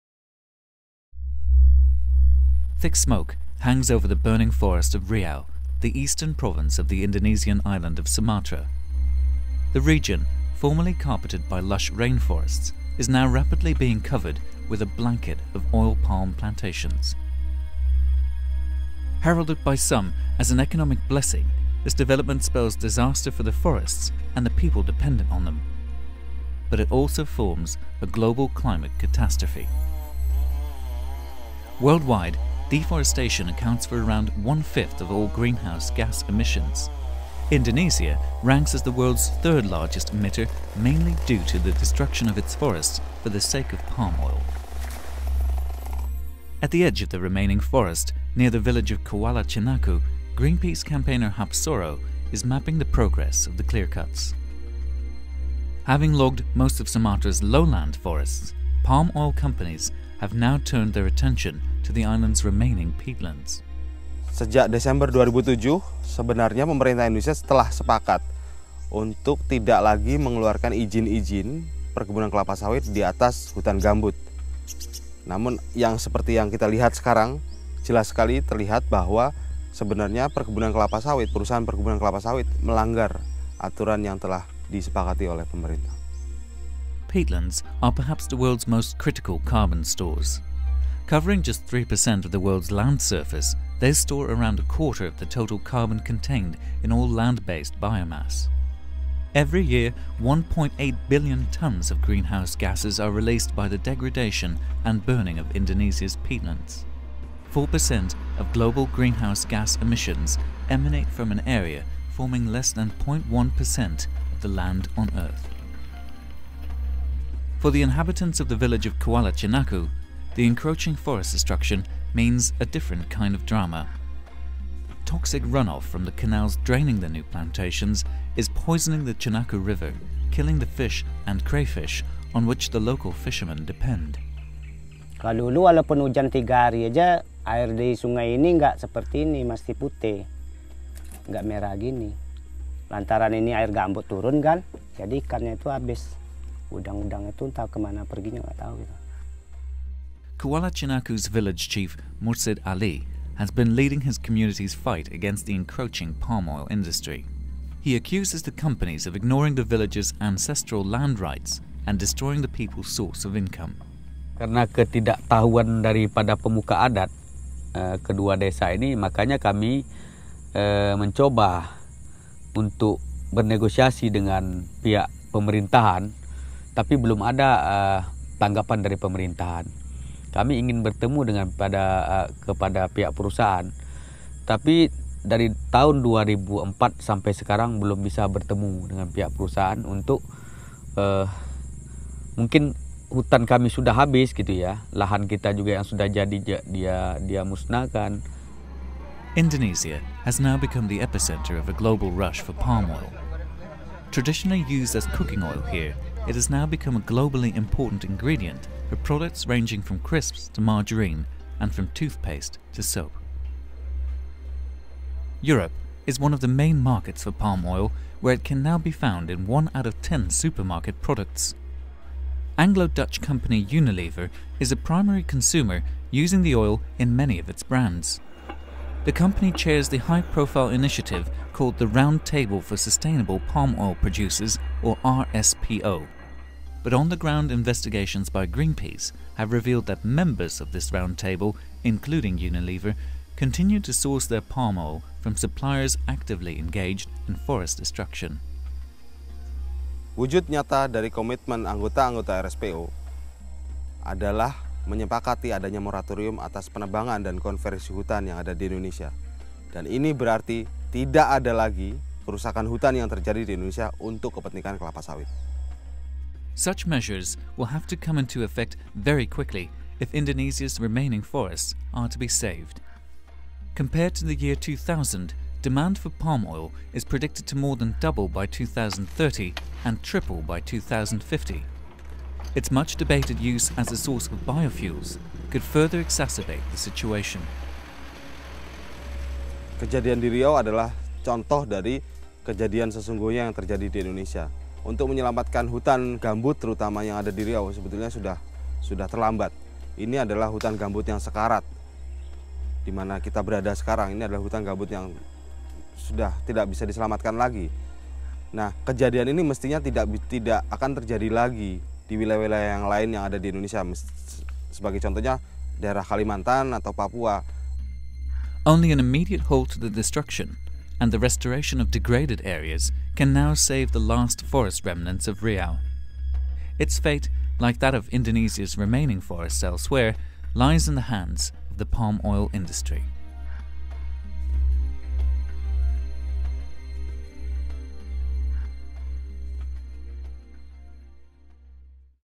Cooking the Climate
The soaring global demand for palm oil is accelerating the destruction of the Indonesian rainforests. A tragedy for the forests and the people who depend on them and a disaster for the global climate.